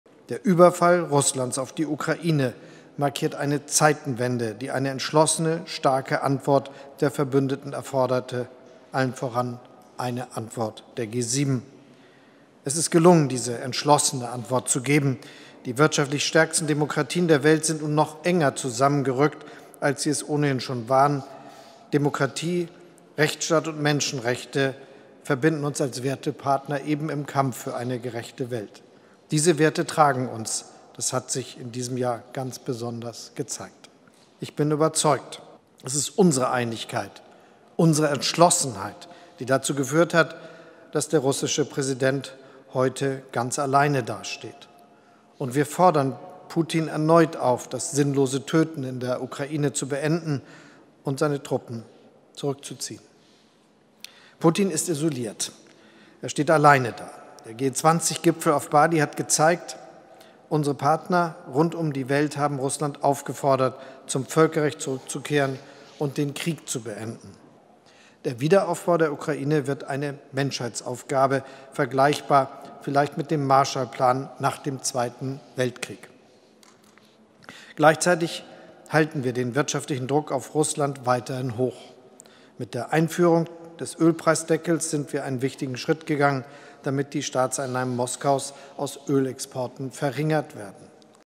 Bundeskanzler Scholz nach G7-Gipfel: „Wir haben Putin isoliert“
Die G7-Staaten sichern der Ukraine ihre weitere Unterstützung im Abwehrkampf gegen Russland zu. Die Staaten stünden nach wie vor fest an der Seite der Ukraine, sagte Bundeskanzler Olaf Scholz (SPD) am Montagabend in Berlin nach einer Schaltkonferenz mit den anderen Staats- und Regierungschefs und dem ukrainischen Präsidenten Wolodymyr Selenskyj. Man werde die Ukraine unterstützen „solange, wie es nötig ist“. Gleichzeitig halte man den wirtschaftlichen Druck auf Russland hoch.